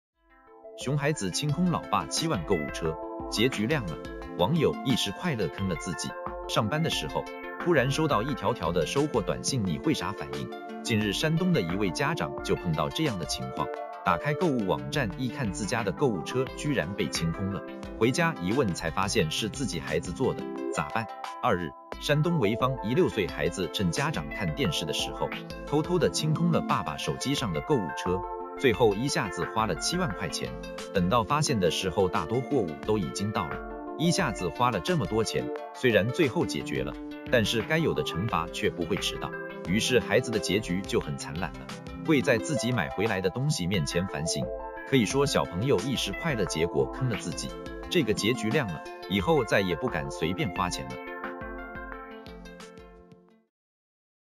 熊孩子清空老爸7万购物车,结局亮了,网友：一时快乐坑了自己
➤Thanks收看！